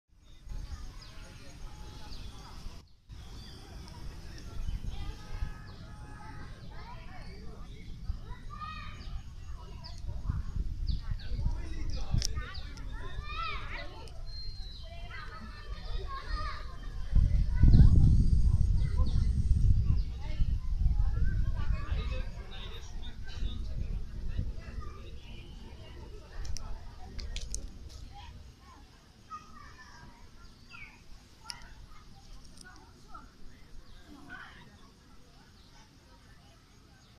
জিন্দা পার্কের হিজল ফুল
রূপগঞ্জের জিন্দা পার্ক ভ্রমণ। ২৪ আগস্ট ২০১৮ শুক্রবার। সঙ্গীসাথী বিহান, নৈঋত, সুমন ভাই, আরিফা, মিথিলা, মকসু, সুমি